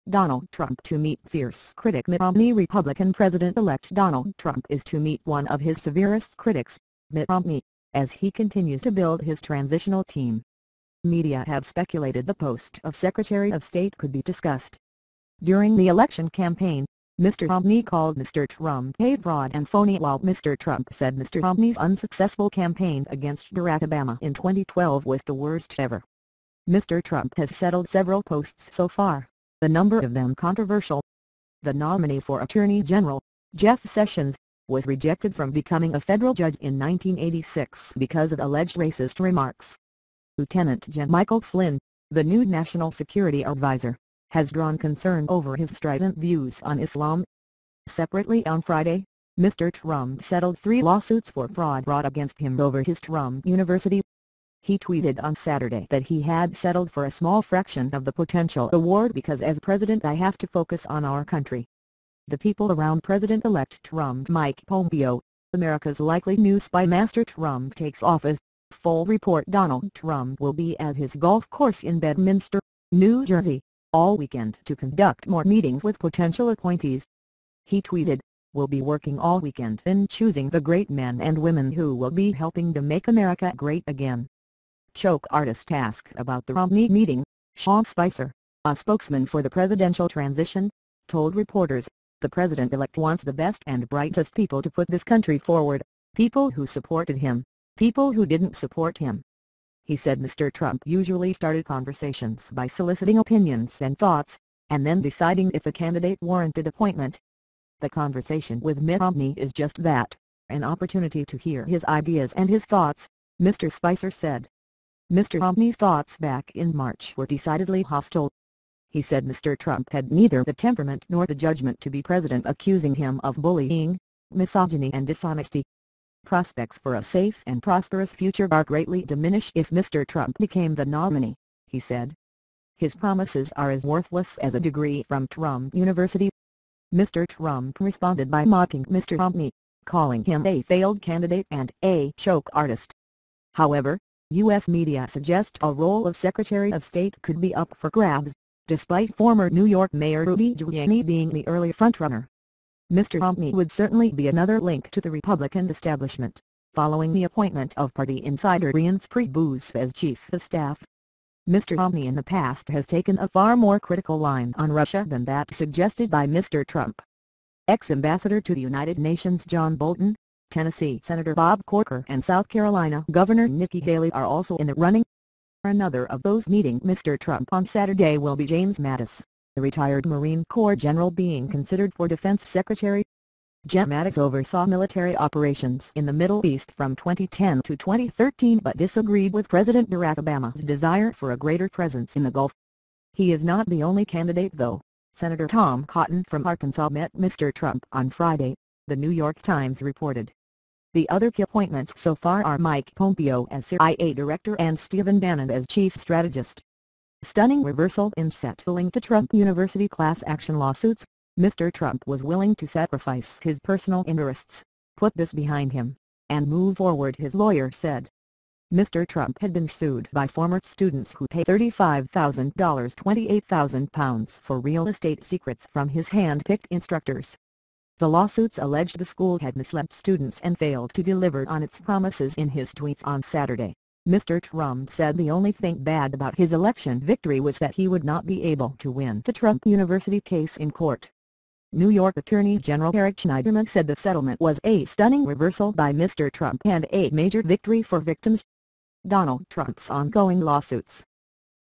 Donald Trump to meet fierce critic Mitt Romney
Republican President-elect Donald Trump is to meet one of his severest critics, Mitt Romney, as he continues to build his transitional team.
Media have speculated the post of secretary of state could be discussed.
During the election campaign, Mr Romney called Mr Trump a "fraud" and "phony", while Mr Trump said Mr Romney's unsuccessful campaign against Barack Obama in 2012 was "the worst ever".
Mr Trump has settled several posts so far, a number of them controversial.
The nominee for attorney general, Jeff Sessions, was rejected from becoming a federal judge in 1986 because of alleged racist remarks.
Lt Gen Michael Flynn, the new national security adviser, has drawn concern over his strident views on Islam.
Separately on Friday, Mr Trump settled three lawsuits for fraud brought against him over his Trump University.
He tweeted on Saturday that he had settled "for a small fraction of the potential award because as President I have to focus on our country".
The people around President-elect Trump
Mike Pompeo - America's likely new spymaster
Trump takes office: Full report
Donald Trump will be at his golf course in Bedminster, New Jersey, all weekend to conduct more meetings with potential appointees.
He tweeted: "Will be working all weekend in choosing the great men and women who will be helping to MAKE AMERICA GREAT AGAIN!"
'Choke artist'
Asked about the Romney meeting, Sean Spicer, a spokesman for the presidential transition, told reporters, "The president-elect wants the best and brightest people to put this country forward: people who supported him, people who didn't support him."
He said Mr Trump usually started conversations by soliciting opinions and thoughts, and then deciding if a candidate warranted appointment.
"The conversation with Mitt Romney is just that: an opportunity to hear his ideas and his thoughts," Mr Spicer said.
Mr Romney's thoughts back in March were decidedly hostile.
He said Mr Trump had neither "the temperament nor the judgement to be president", accusing him of bullying, misogyny and dishonesty.
"Prospects for a safe and prosperous future are greatly diminished" if Mr Trump became the nominee, he said.
"His promises are as worthless as a degree from Trump University."
Mr Trump responded by mocking Mr Romney, calling him a "failed candidate" and a "choke artist".
However, US media suggest the role of secretary of state could be up for grabs, despite former New York mayor Rudy Giuliani being the early frontrunner.
Mr Romney would certainly be another link to the Republican establishment, following the appointment of party insider Reince Priebus as chief of staff.
Mr Romney in the past has taken a far more critical line on Russia than that suggested by Mr Trump.
Ex-ambassador to the United Nations John Bolton, Tennessee Senator Bob Corker and South Carolina Governor Nikki Haley are also in the running.
Another of those meeting Mr Trump on Saturday will be James Mattis, a retired Marine Corps general being considered for defence secretary.
Gen Mattis oversaw military operations in the Middle East from 2010 to 2013 but disagreed with President Barack Obama's desire for a greater presence in the Gulf.
He is not the only candidate though - Senator Tom Cotton from Arkansas met Mr Trump on Friday, the New York Times reported.
The other key appointments so far are Mike Pompeo as CIA director and Stephen Bannon as chief strategist.
'Stunning reversal'
In settling the Trump University class-action lawsuits, Mr Trump "was willing to sacrifice his personal interests, put this behind him, and move forward", his lawyer said.
Mr Trump had been sued by former students who paid $35,000 (£28,000) for real estate "secrets" from his "hand-picked" instructors.
The lawsuits alleged the school had misled students and failed to deliver on its promises
In his tweets on Saturday, Mr Trump said the only thing bad about his election victory was that he would not be able to win the Trump University case in court.
New York Attorney General Eric Schneiderman said the settlement was a "stunning reversal" by Mr Trump and a "major victory" for victims.
Donald Trump's ongoing lawsuits